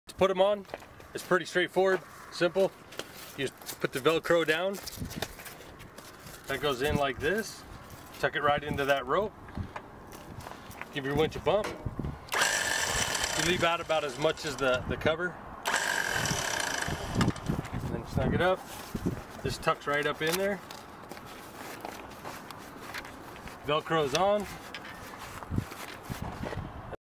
How to install the drum winch cover
Installing the drum barrel winch cover to protect the rope and still show off the winch.